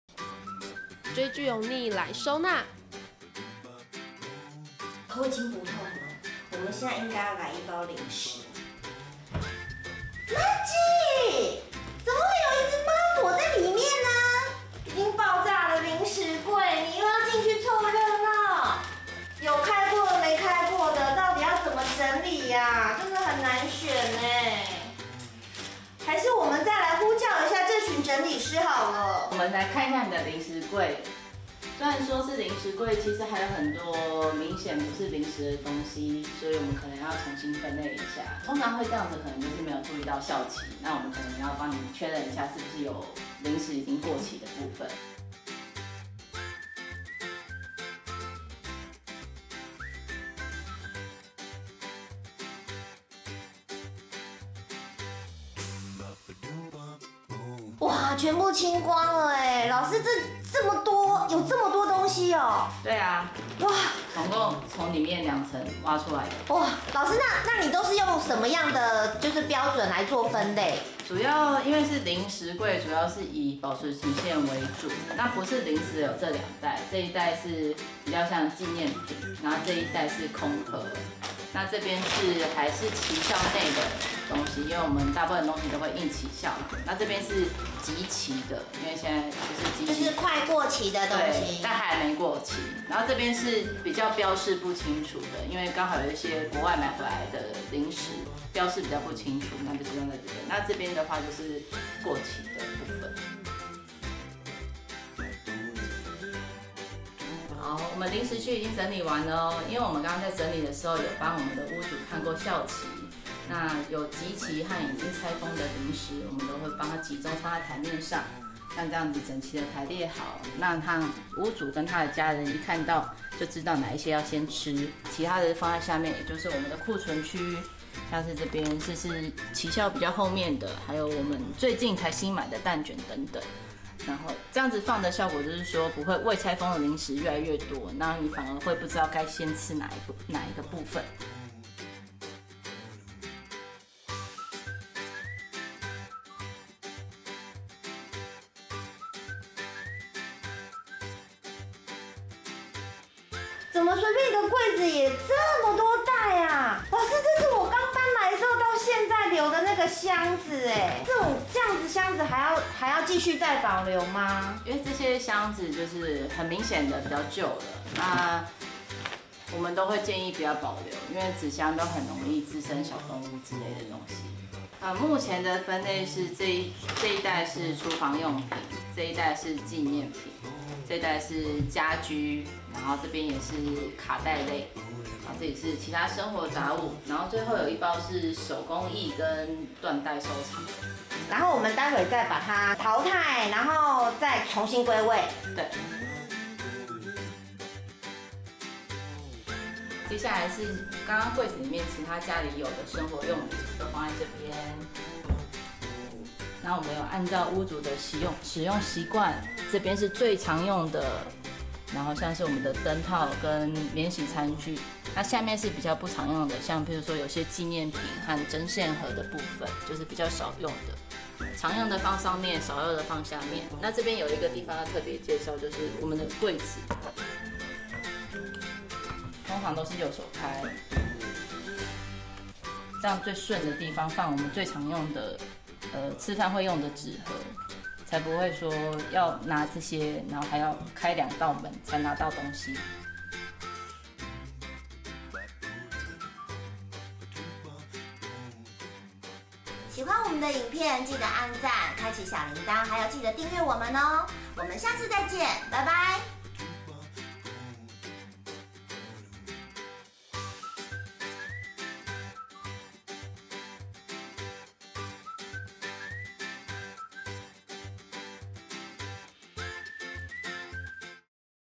追劇歐逆帶妳輕鬆打造零食櫃｜追劇吃零食才開心！
🍡追劇時你最喜歡搭配哪種零食🍡
《追劇歐逆》教妳輕鬆打造零食櫃
讓妳美食在手，追劇不間斷🏃‍♀️🏃🏃‍♂️
整理有序的零食櫃再也不怕放到過期啦！
感謝 這群整理師